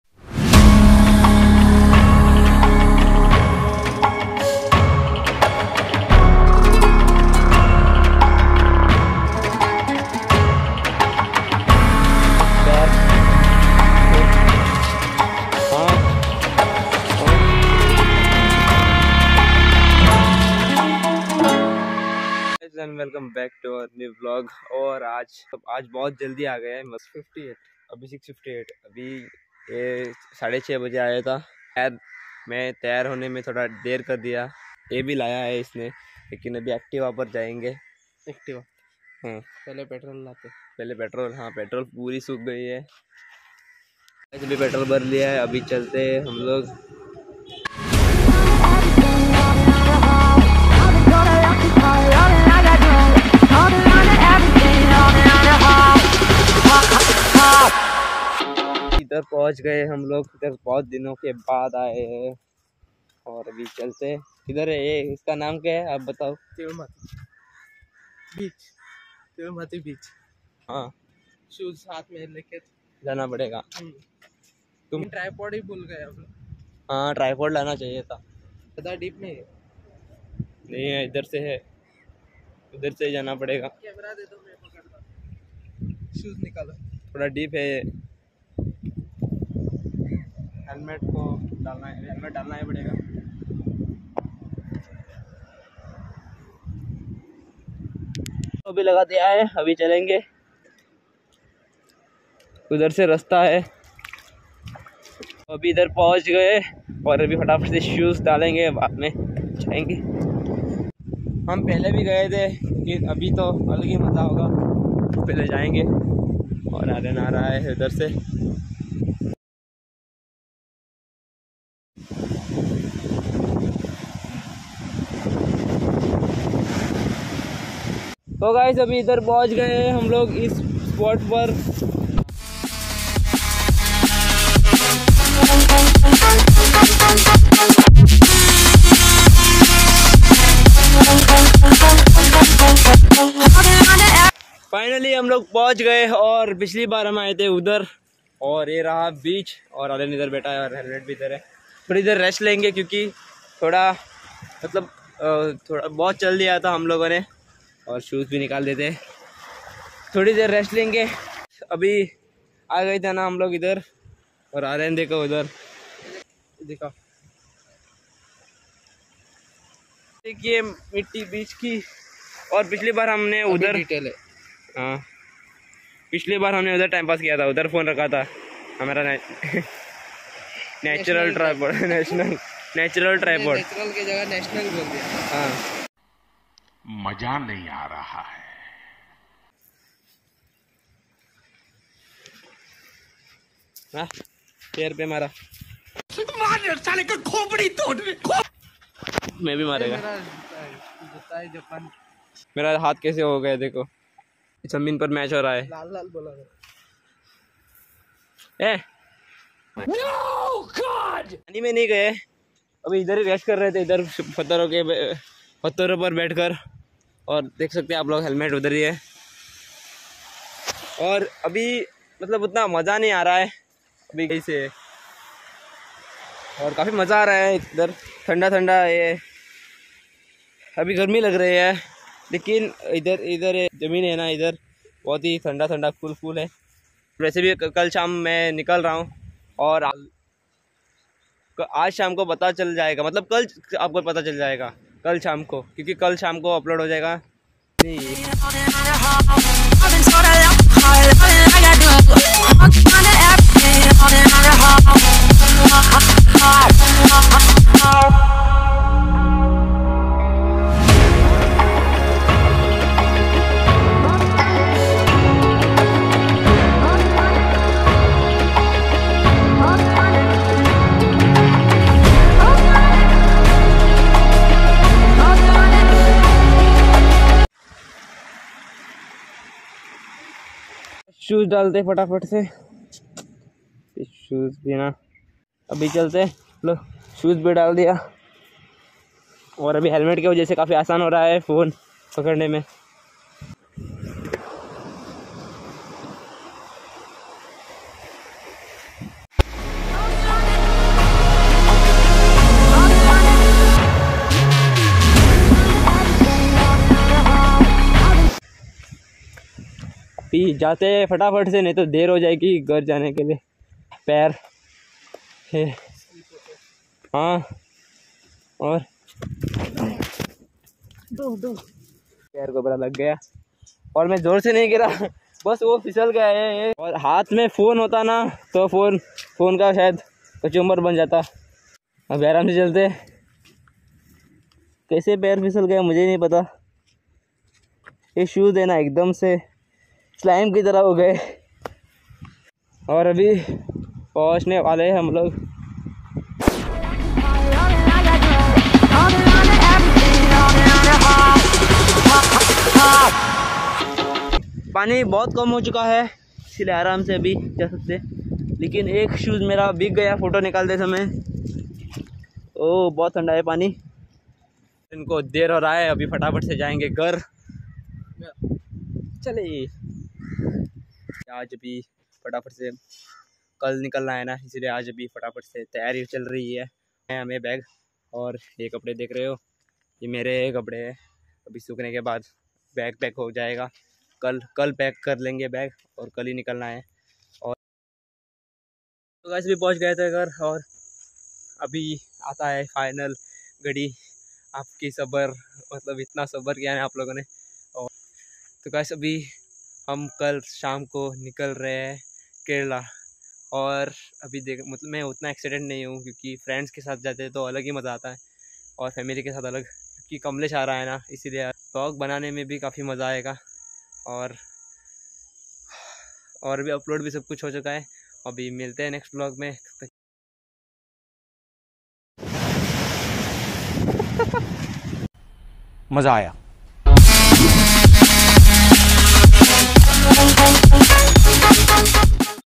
Tilmati beach karwar 😍 🏖️🪨. finally trip revealed / vlog 82
The Tilmati beach in Karwar is located in Uttara Kannada district of Karnataka. The sand here looks like black sesame, which is why the beach is named Tilmati. The name comes from the Konkani word 'tillu' which means sesame seeds and 'matti' which means soil .

editing device:
👉Vivo V21e 5G

mic:
 👉boya BY M1 pro

thumbnail editing app:
👉 PicsArt